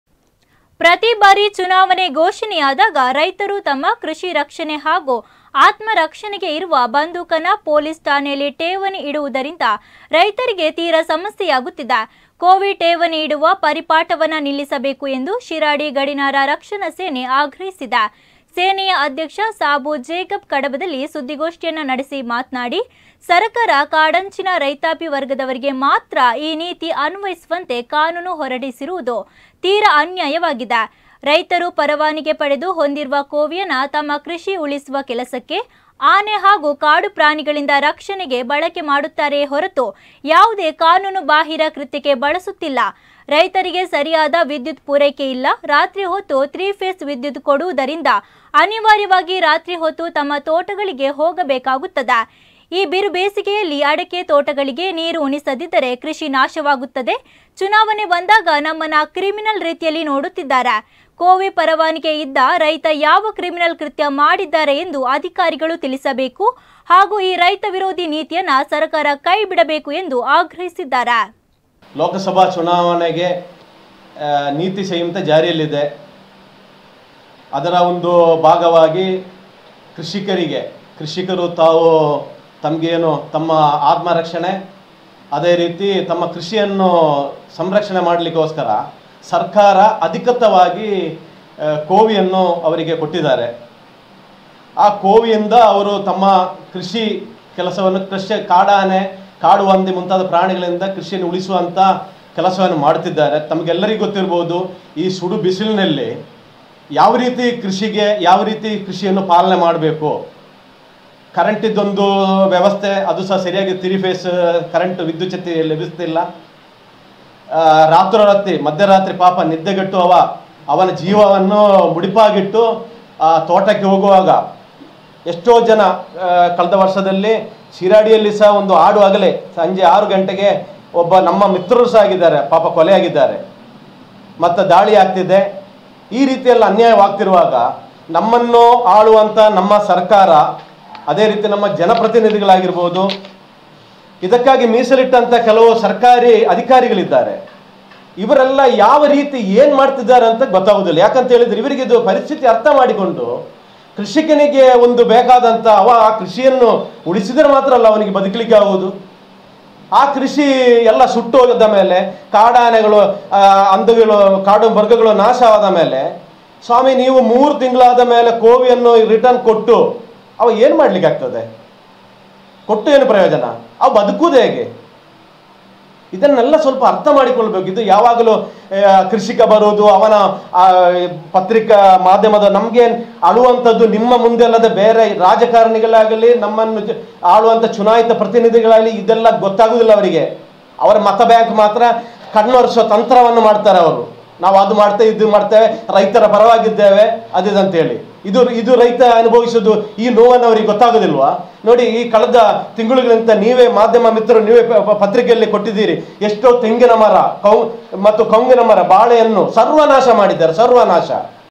"ಚುನಾವಣೆ ಸಂದರ್ಭದಲ್ಲಿ ಕೋವಿ ಠೇವಣಿ ಇಡುವುದನ್ನು ಕೈಬಿಡಬೇಕು": ಶಿರಾಡಿಯ ಗಡಿನಾಡ ರಕ್ಷಣಾ ಸೇನೆ ಆಗ್ರಹ
"ಚುನಾವಣಾ ಹಿನ್ನೆಲೆ ರೈತರು ಕೋವಿಗಳನ್ನು ಪೊಲೀಸ್ ಠಾಣೆಗಳಲ್ಲಿ ಠೇವಣಿಸಬೇಕು" ; ಈ ಬಗ್ಗೆ ಸೇನೆಯ ಅಧ್ಯಕ್ಷ  ಸಾಬೂ ಜೇಕಬ್ ಆಕ್ರೋಶ; "ಕೋವಿ ಠೇವಣಿ ಇಡುವ ಪರಿಪಾಠವನ್ನು ನಿಲ್ಲಿಸಬೇಕು"
"ಕೋವಿ ಪರವನಗಿ ಪಡೆದ ರೈತ ಯಾವ ಕೃತ್ಯ ನಡೆಸಿದ್ದಾನೆ"; ಅಧಿಕಾರಿಗಳಿಗೆ ಪ್ರಶ್ನೆಗಳ ಸುರಿಮಳೆ